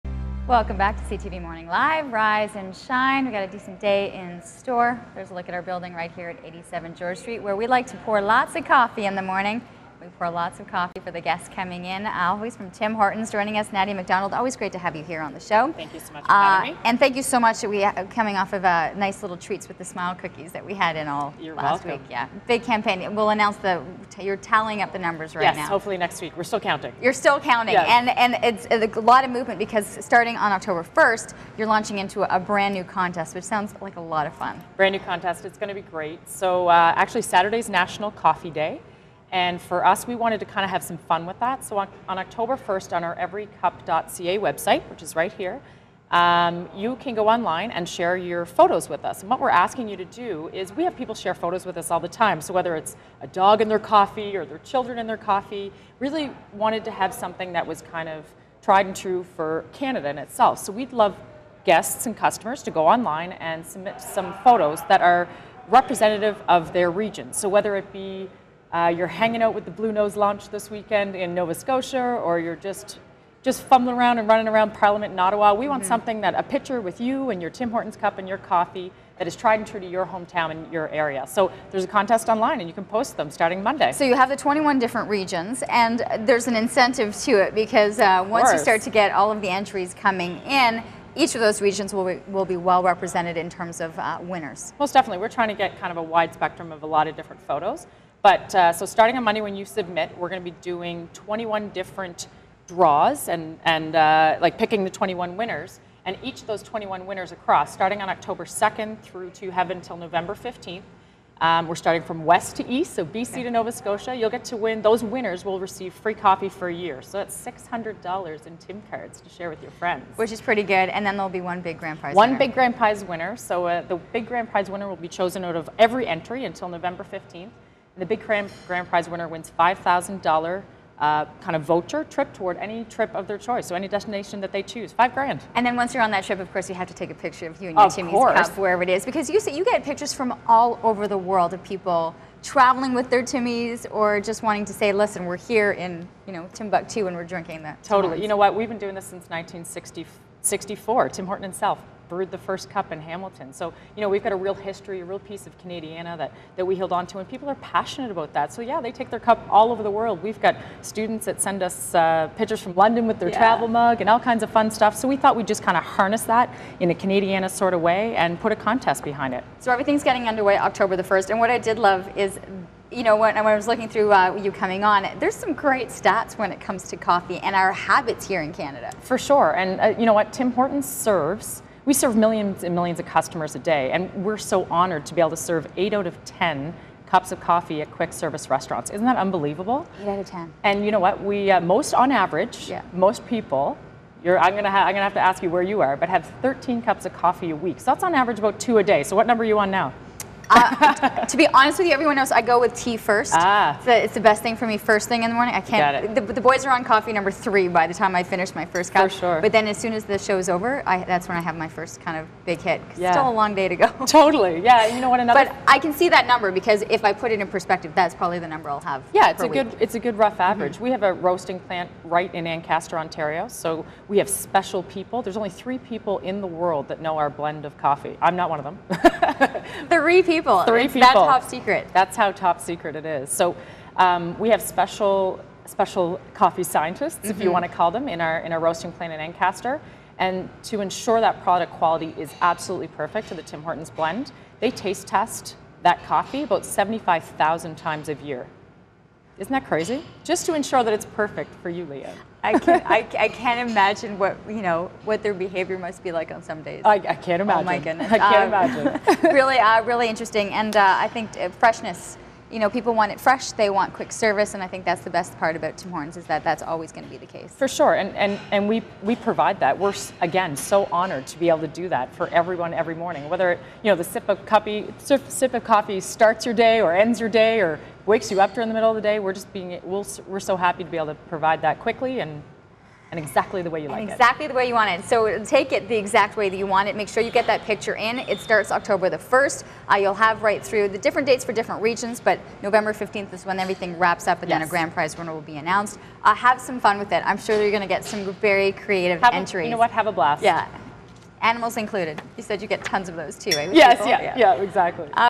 Tim Hortons Tims Tour
Tim Hortons Regional Marketing Manager Nadia MacDonald dropped by CTV Morning Live to discuss the Tims Tour. Starting next week, Tim Hortons is launching an online photo sharing contest giving Canadians a chance to win free coffee for a year or the grand prize of a $5,000 trip.